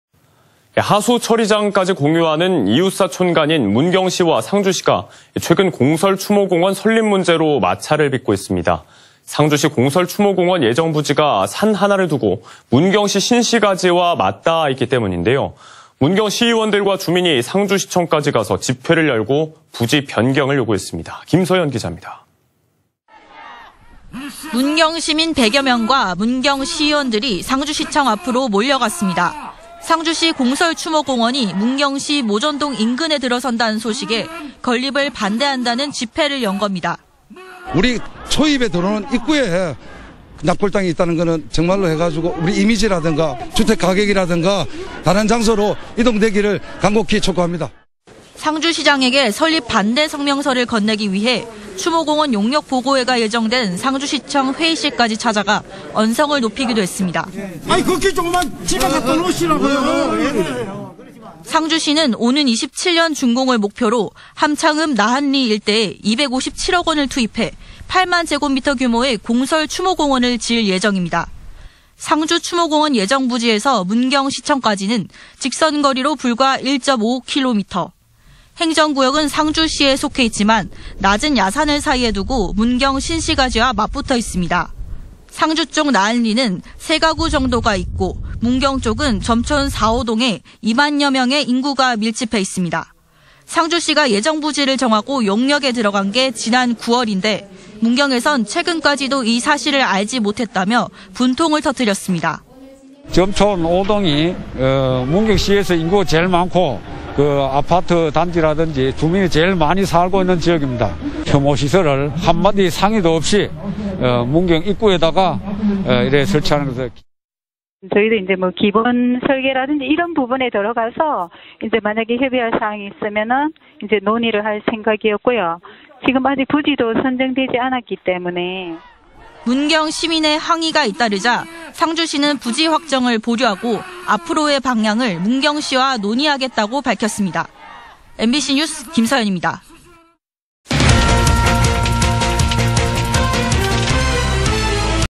R]상주 추모공원 설립 두고 문경과 이웃간 갈등 / 안동MBC
2021/12/27 17:59:38    작성자 : 김서현

◀ＡＮＣ▶
하수처리장까지 공유하는 이웃사촌 간인
문경시와 상주시가,
최근 공설추모공원 설립 문제로
마찰을 빚고 있습니다.

상주시 공설 추모공원 예정 부지가
산 하나를 두고 문경시 신시가지와
맞닿아있기 때문인데요,

문경시의원들과 주민이
상주시청까지 가서 집회를 열고
부지 변경을 요구했습니다.

김서현 기자
◀ＥＮＤ▶
◀ＶＣＲ▶
문경시민 백 여 명과 문경시의원들이
상주시청 앞으로 몰려갔습니다.

상주시 공설추모공원이
문경시 모전동 인근에 들어선다는 소식에
건립을 반대한다는 집회를 연 겁니다.

◀ＩＮＴ▶진후진 문경시의원
"우리(문경시) 초입에 들어오는 입구에
납골당이 있다는 건 우리 이미지라든가
주택가격이라든가 (떨어질 것이고)
다른 장소로 이동되기를 간곡히 촉구합니다."

상주시장에게 설립 반대 성명서를 건네기 위해,
추모공원 용역 보고회가 예정된
상주시청 회의실까지 찾아가
언성을 높이기도 했습니다.

◀ＳＹＮ▶김창기 문경시의회 의장
"아니, 그렇게 좋으면 집에 갖다놓으시라고요."

상주시는 오는 27년 준공을 목표로
함창읍 나한리 일대에 257억 원을 투입해
8만 제곱미터 규모의 공설 추모공원을
지을 예정입니다.

[C.G]
상주 추모공원 예정부지에서 문경시청까지는
직선거리로 불과 1.5km.
[끝]

행정구역은 상주시에 속해 있지만
낮은 야산을 사이에 두고
문경 신시가지와 맞붙어 있습니다.

상주 쪽 나한리는 3가구 정도가 있고,
문경 쪽은 점촌 4, 5동에 2만여 명의 인구가
밀집해 있습니다.

상주시가 예정부지를 정하고 용역에 들어간 게 지난 9월인데, 문경에선 최근까지도
이 사실을 알지 못했다며 분통을 터뜨렸습니다.

◀ＩＮＴ▶우종식 / 문경시 점촌5동
"점촌5동이 문경시에서 인구가 제일 많고
아파트 단지라든지 주민이 많이 살고 있는
지역입니다. 혐오시설을 한 마디 상의도 없이
문경 입구에 설치하는 건 (문제가 있습니다.)"

◀ＩＮＴ▶천인숙 가족복지과장 / 상주시
"저희도 기본 설계라든지 이런 부분에 들어가서
만약에 협의할 사항이 있으면 (문경시와)
논의할 생각이었고요. 지금 아직 부지도
선정되지 않았기 때문에..."

문경시민의 항의가 잇따르자,
상주시는 부지 확정을 보류하고
앞으로의 방향을 문경시와 논의하겠다고
밝혔습니다.

MBC뉴스 김서현입니다.
(영상취재 최재훈, C.G 이한나)